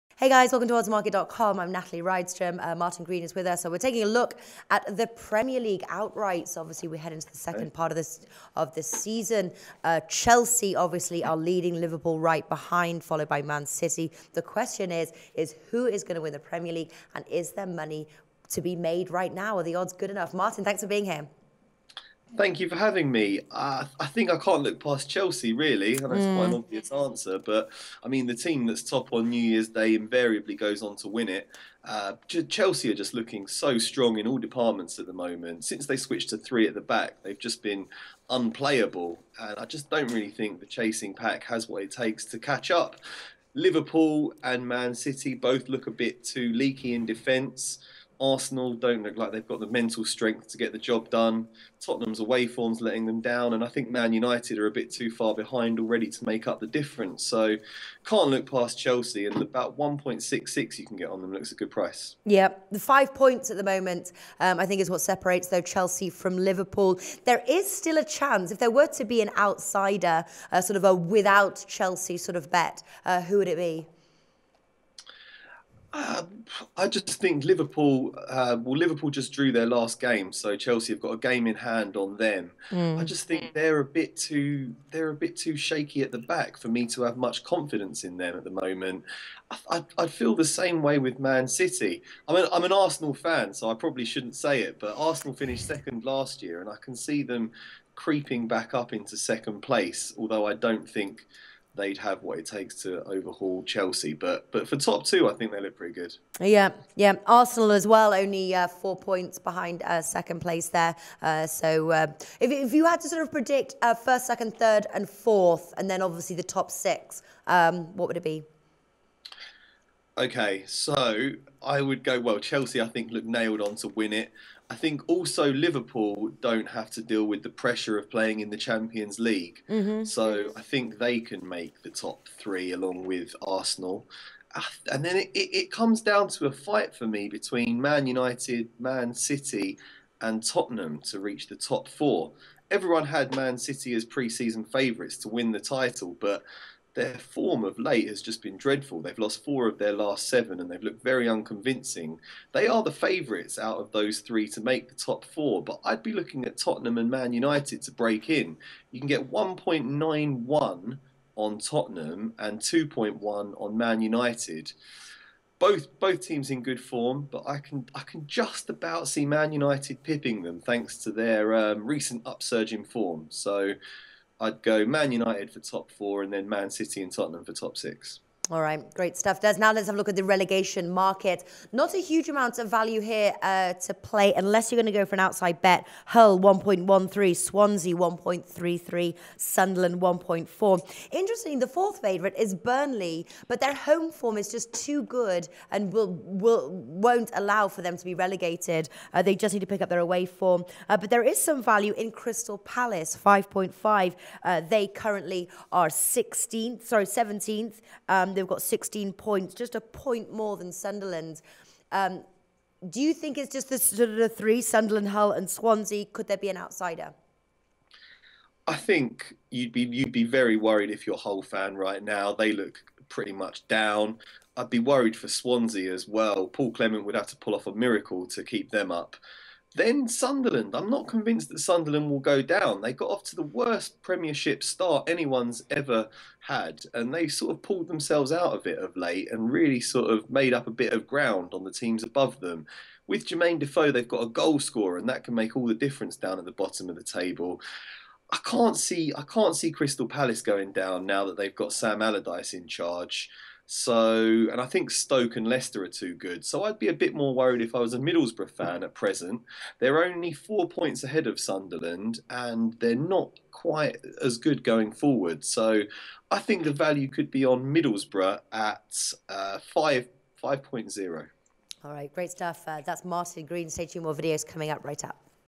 2016-17 Premier League Outrights Betting
Natalie Rydstrom from OddsMarket.com and sports betting writer discuss the updated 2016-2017 Premier League Outrights Betting markets, which team is predicted to win the title, which teams will be relegated and what’s the value bet headed into the New Year.